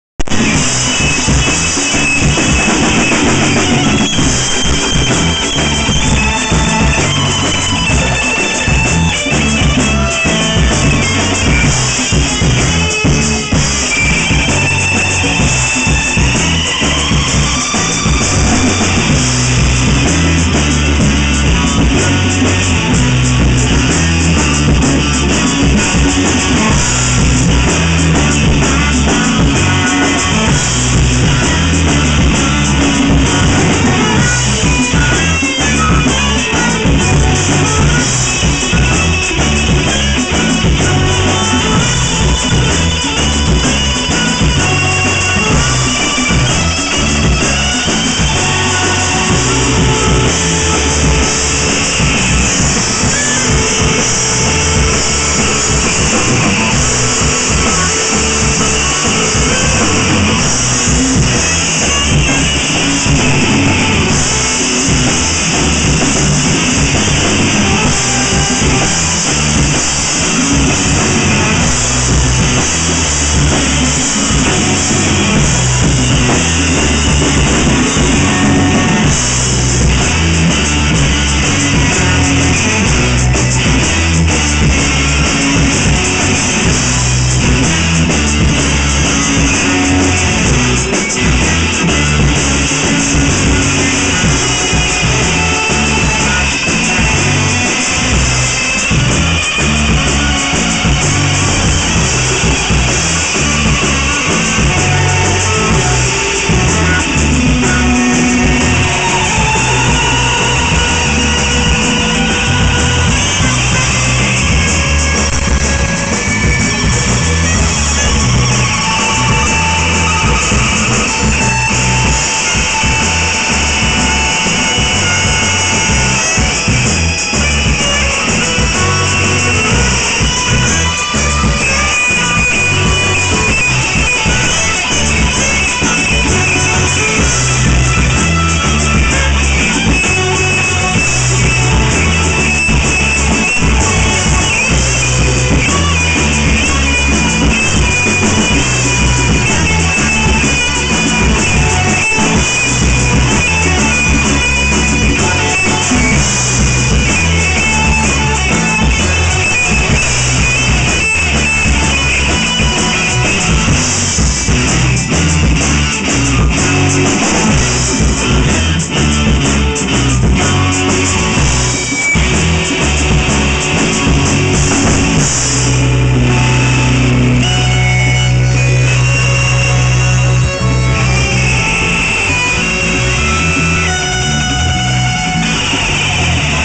The Neo Lebro 1 of 8
John's first show back with the band in years